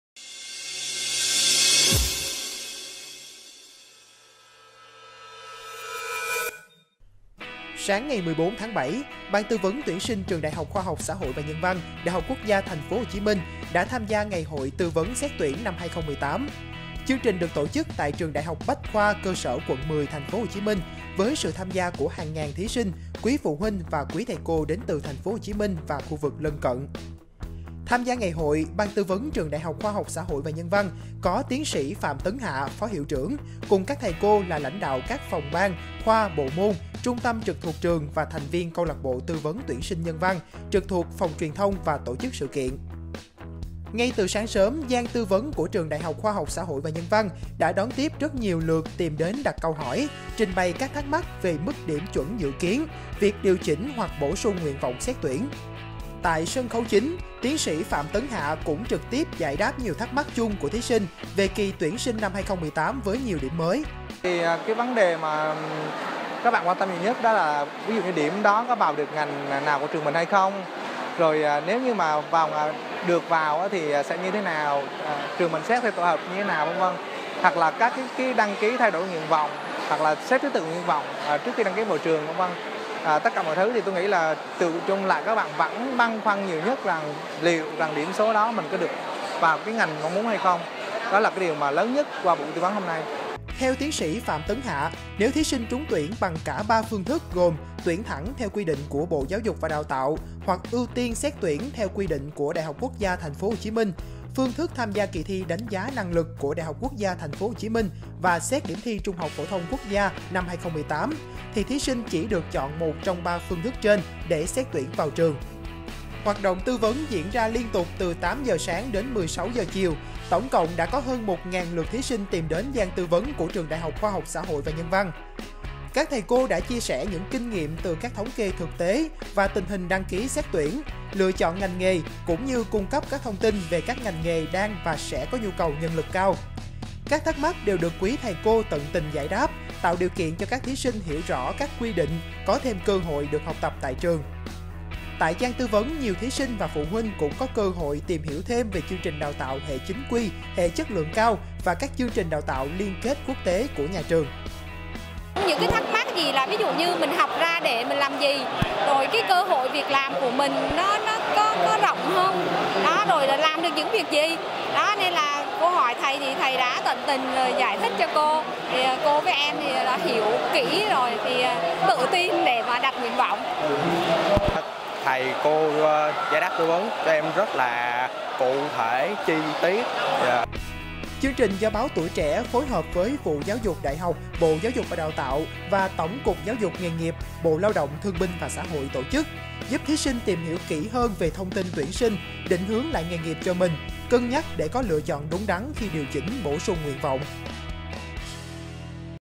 Trúng tuyển nhiều phương thức, phải làm sao?
Ngày 14-7, Ban Tư vấn Tuyển sinh Trường ĐH KHXH&NV đã tham gia Ngày hội Tư vấn Xét tuyển Đại học - Cao đẳng 2018.

Ngày hội có sự góp mặt của TS. Phạm Tấn Hạ - Phó Hiệu trưởng Nhà trường; các thành viên trong Ban Tư vấn Tuyển sinh và lãnh đạo các phòng ban, trung tâm trực thuộc Trường.

Thực hiện: THÚY AN - QUỲNH NHƯ